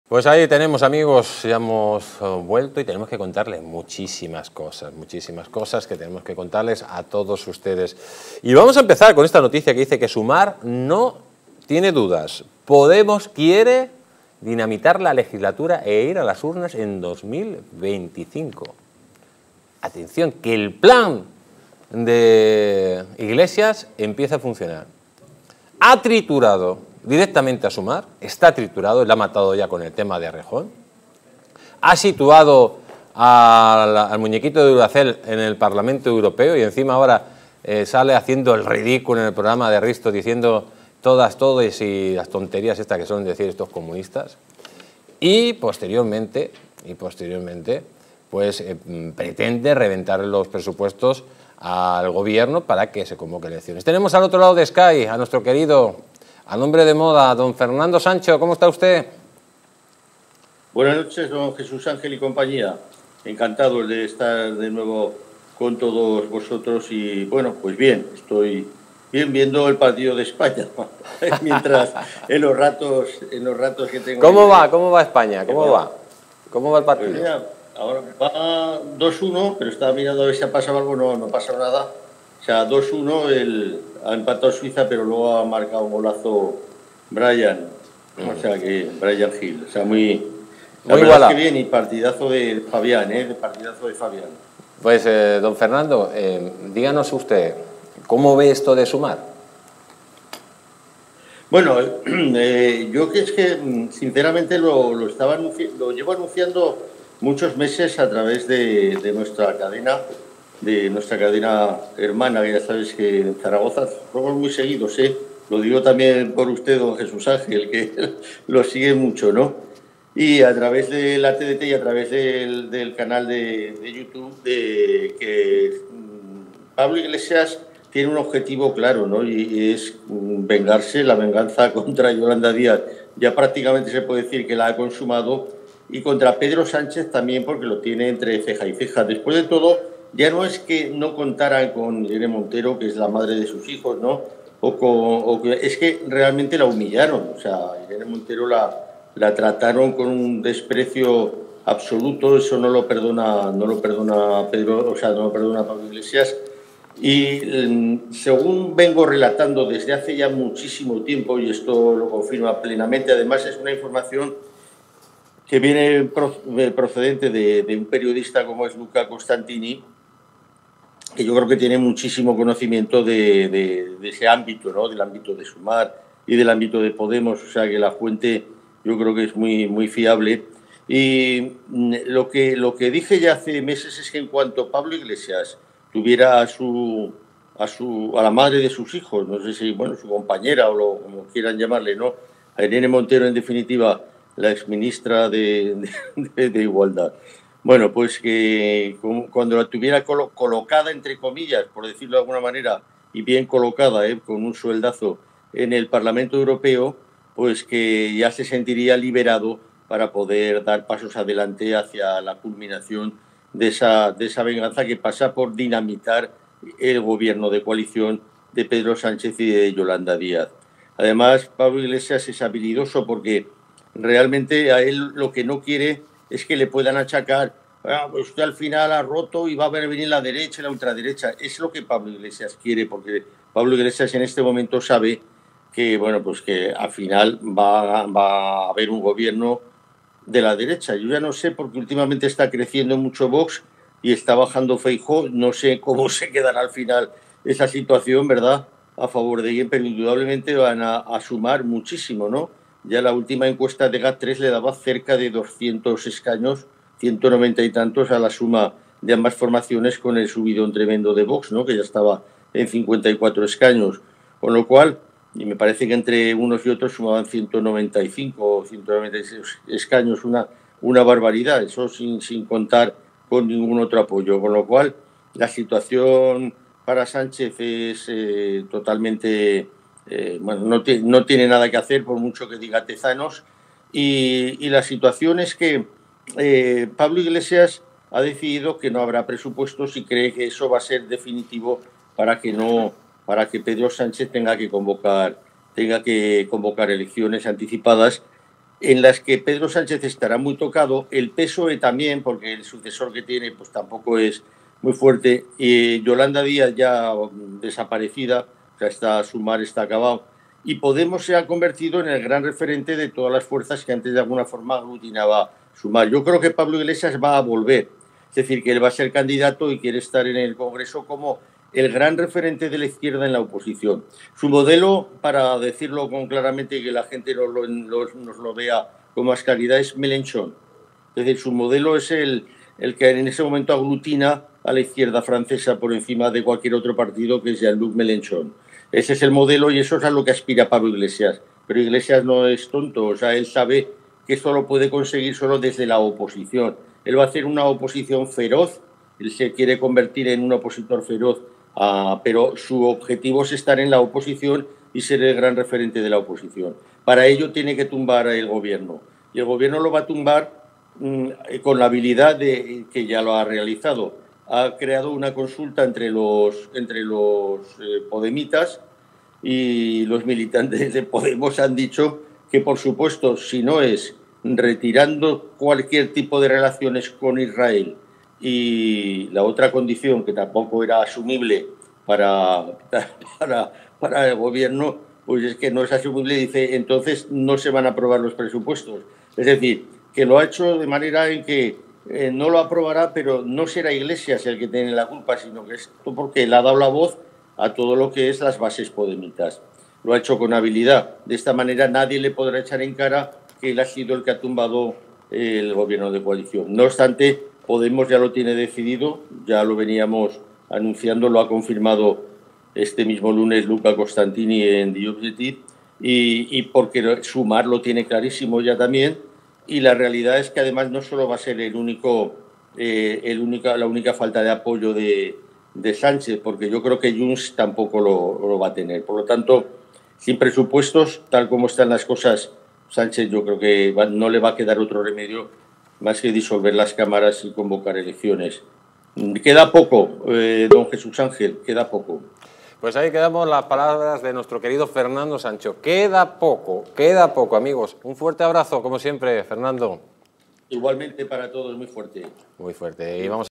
BRUTAL🔴Sumar no tiene dudas: Podemos quiere dinamitar la legislatura e ir a las urnas en 2025🔴
En este video, Sumar no tiene dudas: Podemos quiere dinamitar la legislatura e ir a las urnas en 2025. ¡No te pierdas este análisis en Distrito TV! 🔴